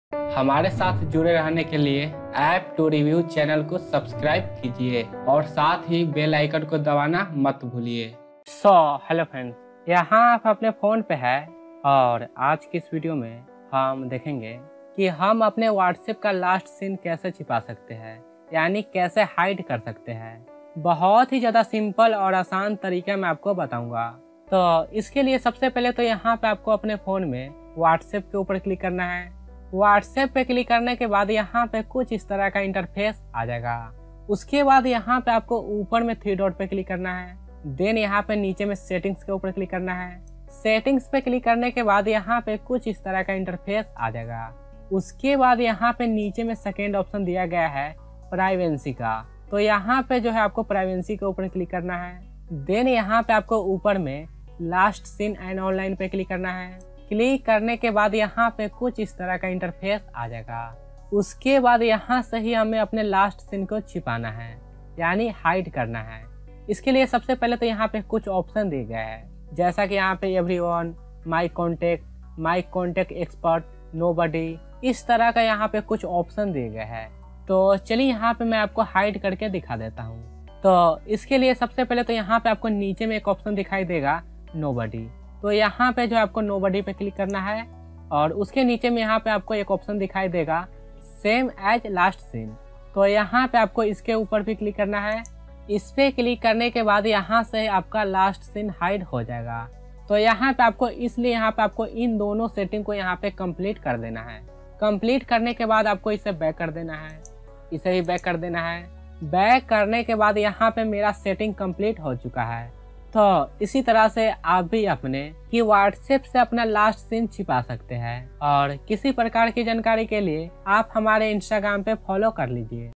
Whatsapp last seen hide kaise kare | Whatsapp par last seen kaise chupaye | 2024
kya aap bhi apne whatsapp se apna last seen hide karna chahte hain to phir ye video dekhte rahiye.